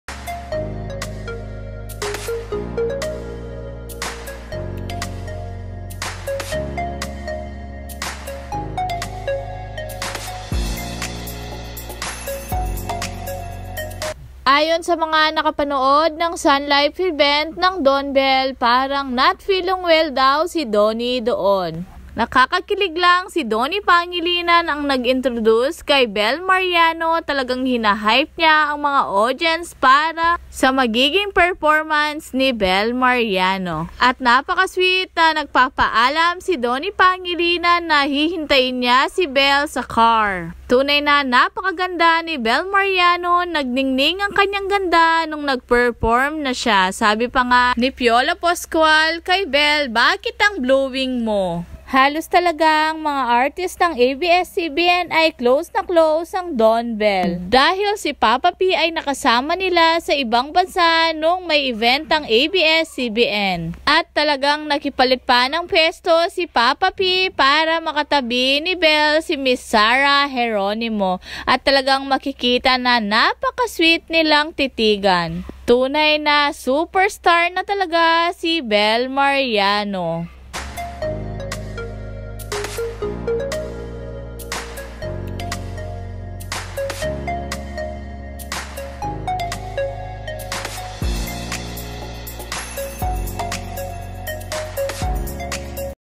GRABE ANG SIGAWAN NG MGA FANS SA GINAWA NI DONNY P. SA SUNFIFE EVENT NILA
Dahil umattend sila ng sunlife event ang donbelle talaga kitang kita ang kanilang kinang ng kanilang chemistry ayon kay piolo pascual tinanong niya si belle m. Bakit napaka blomming niya